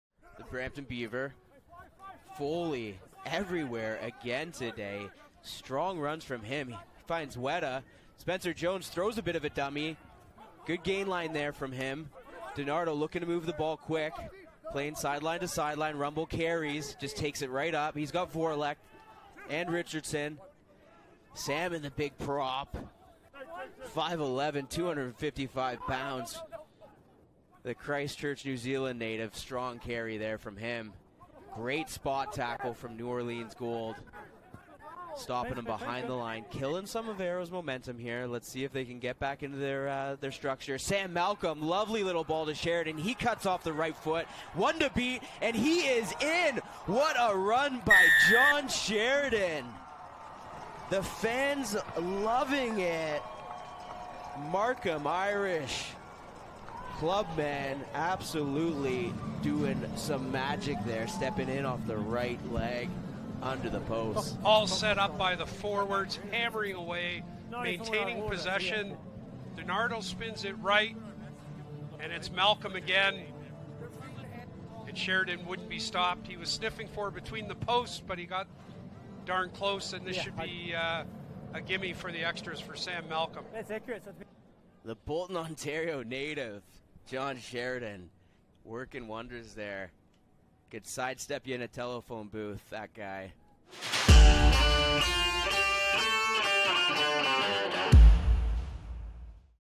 Toronto Arrows | John Sheridan Try vs. NOLA Gold (Apr. 16, 2022)
John Sheridan danced around a couple of would-be tacklers to work his way across the whitewash.